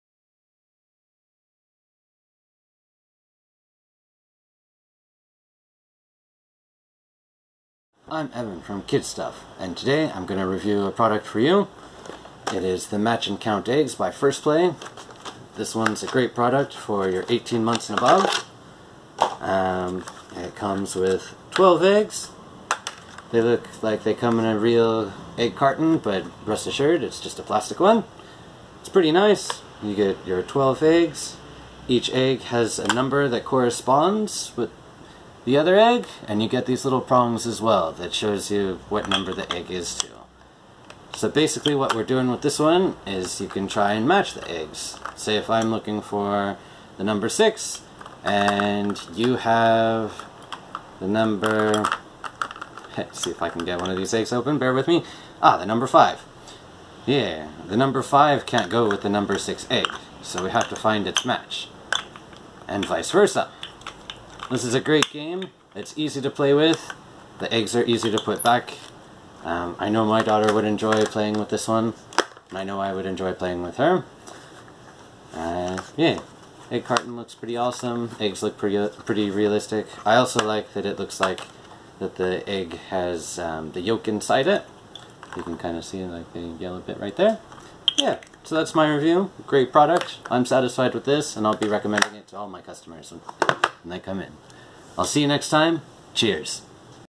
First Play Match & Count Eggs
The First Play Match & Count Eggs is the ideal game to have fun with numbers for kids of ages 18+ and above!

It has a dozen ways to learn and play by joining the pegs and holes or matching the numbers for classic counting fun!

Watch the video to know more about this game and the impact it has on your little one's numerical development.